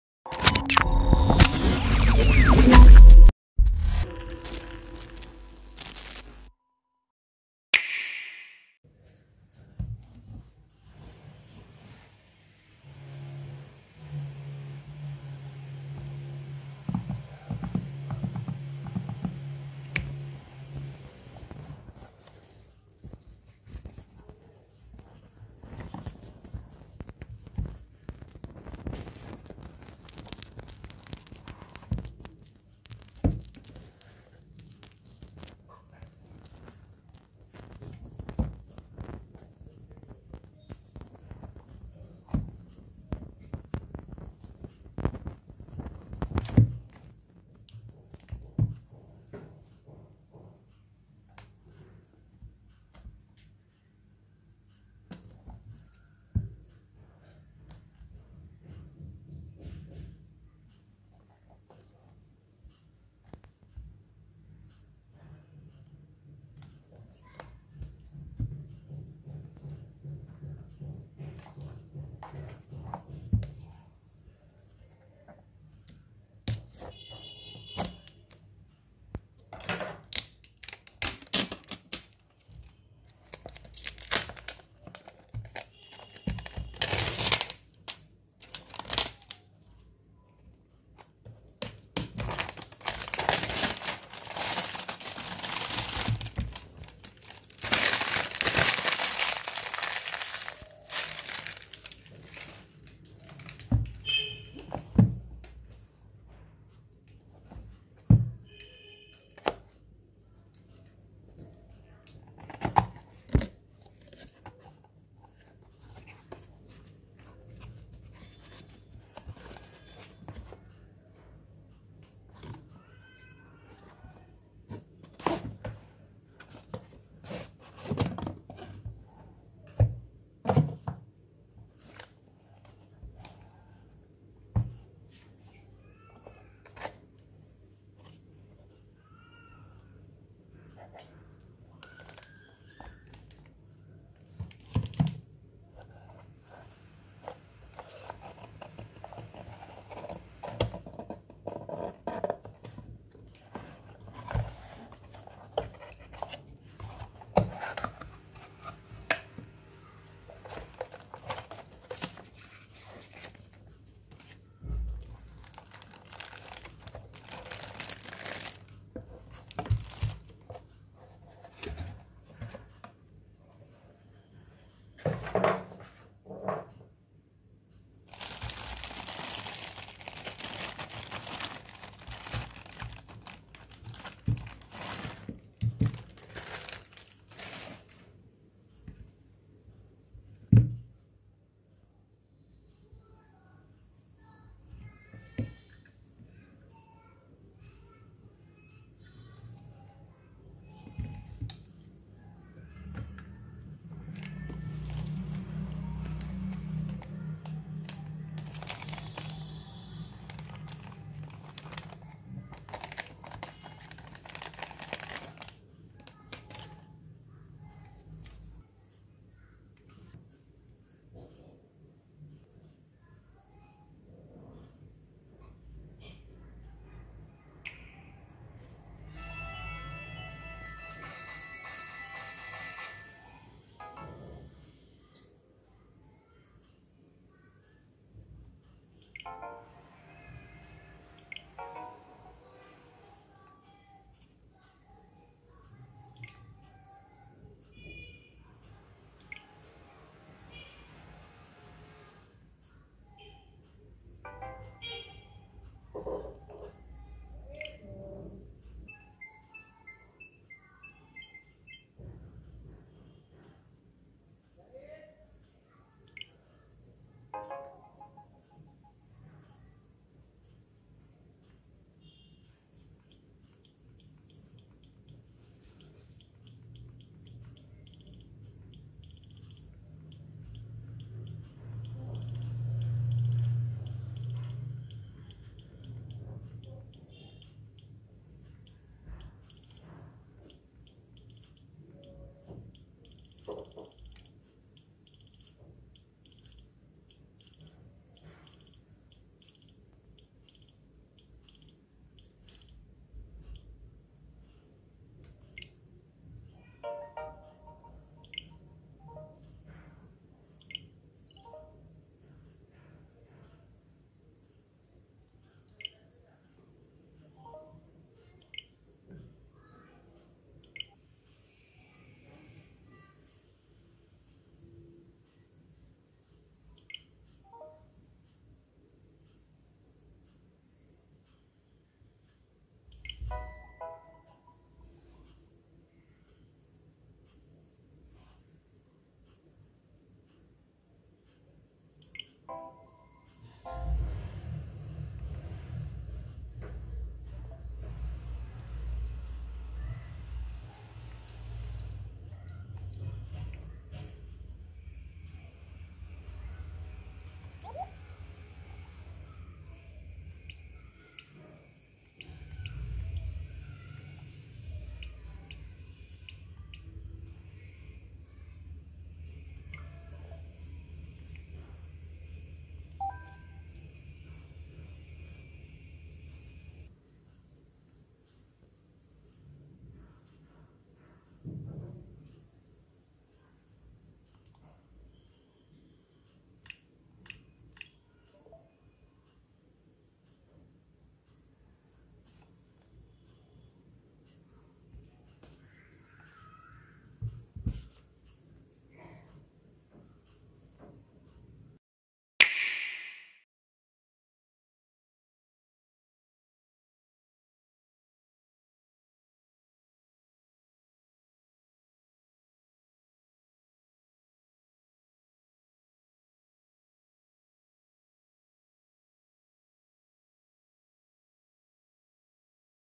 Nintendo Switch Lite Unboxing and Setup (Zacian and Zamazenta Edition) 2021
What do you think about the Nintendo Switch Lite?

it’s gorgeous!.